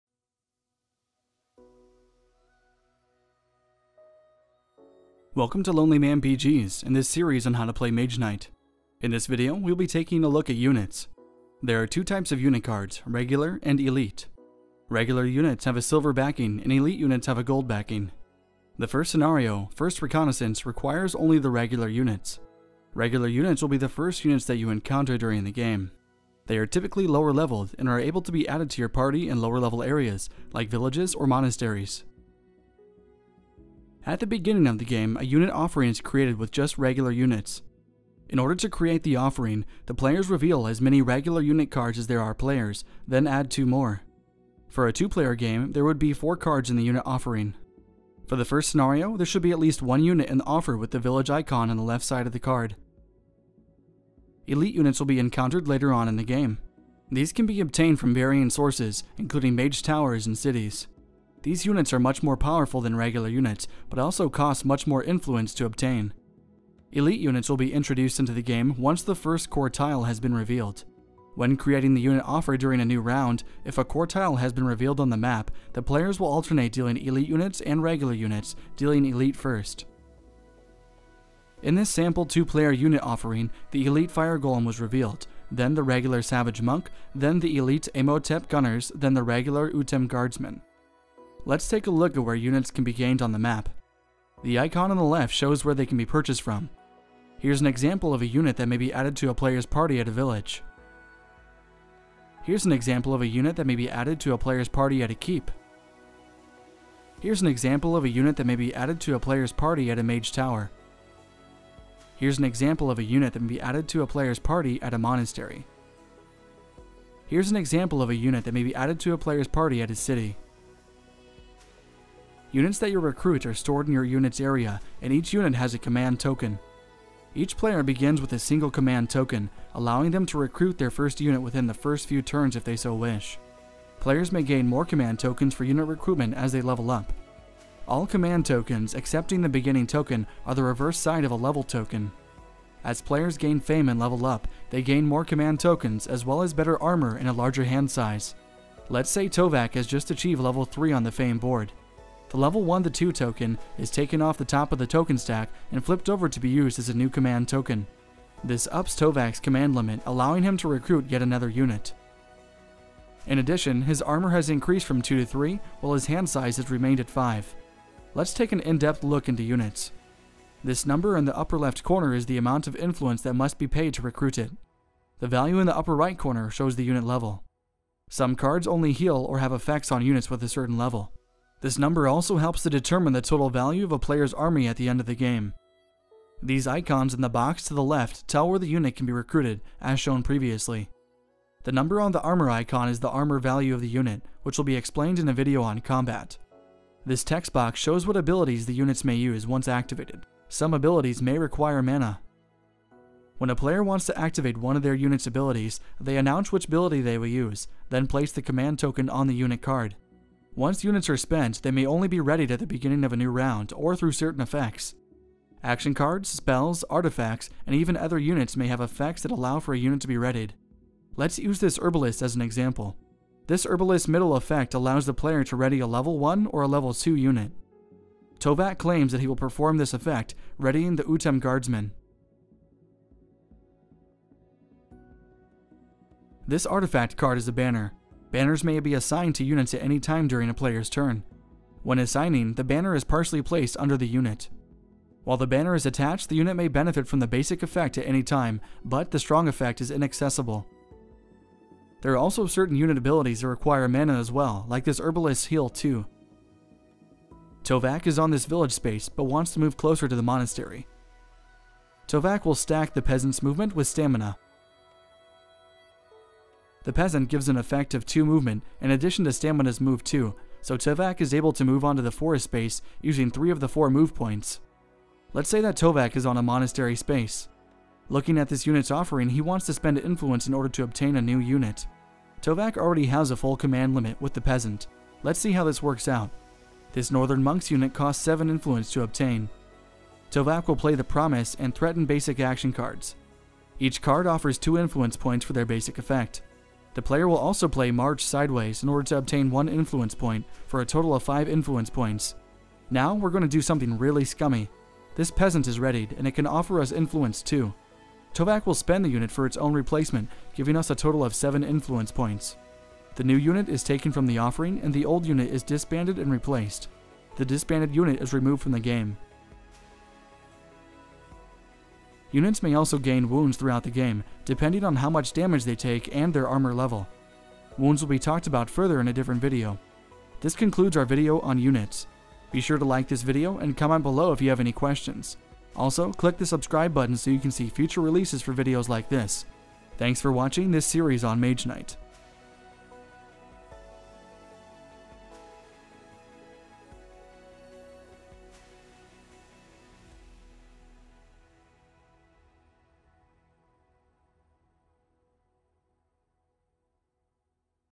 Mage Knight | How to Play | Units
Welcome to this video in the Mage Knight tutorial series. In this video, we will be learning about Units--what they can do and how players obtain them.